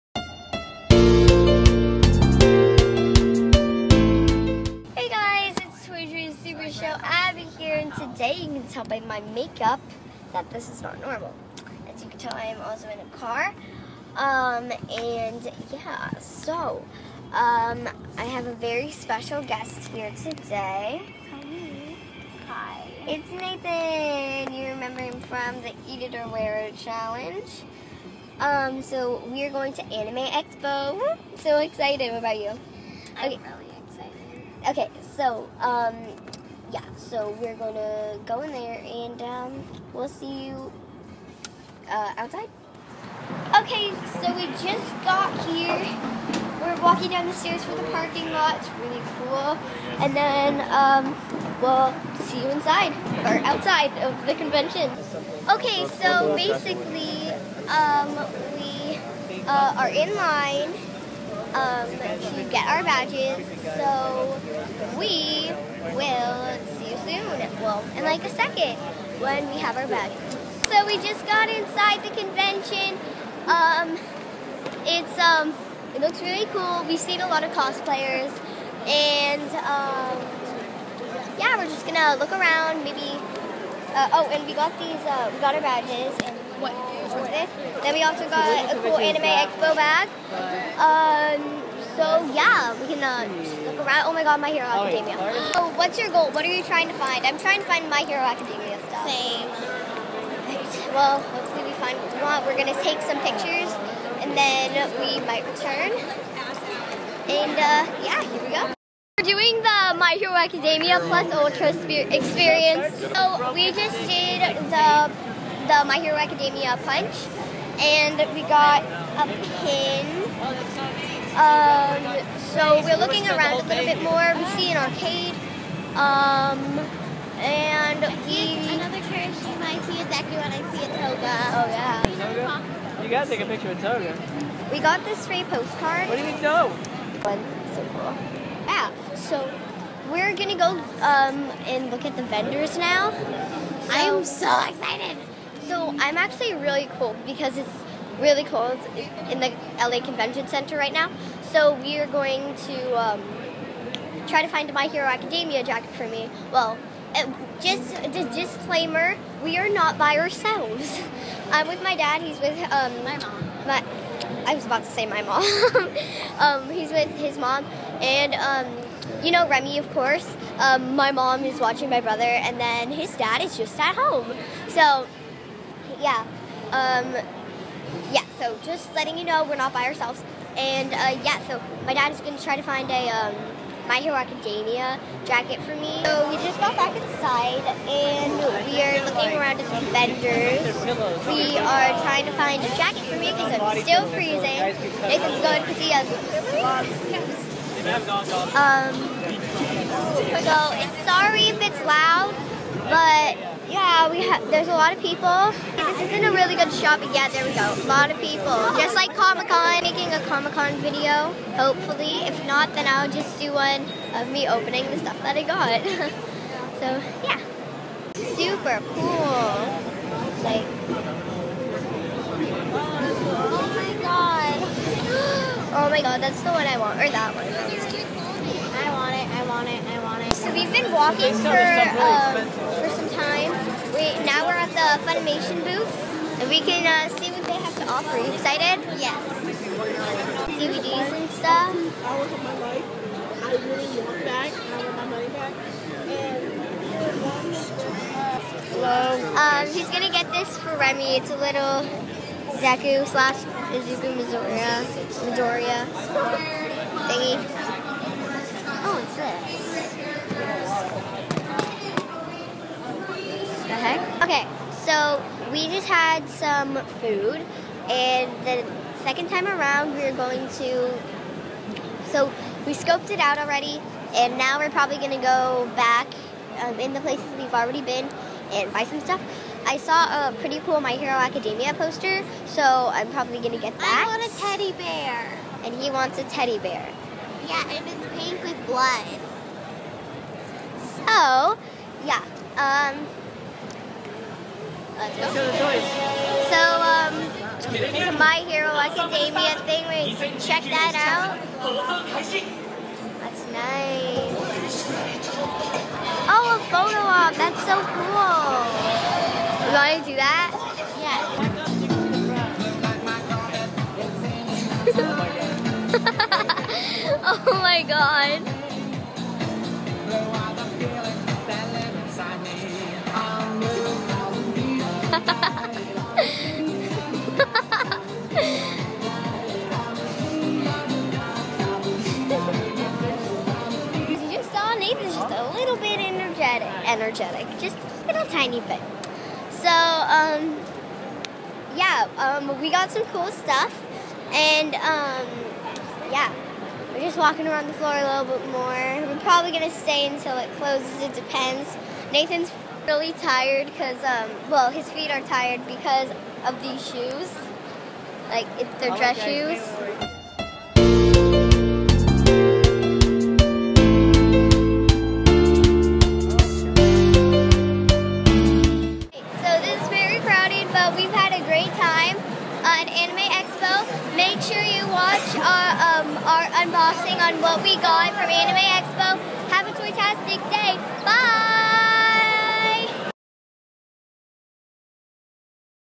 ANIME EXPO 2019
Abby and her pal Nathan hit the 2019 Anime Expo in search of goodies.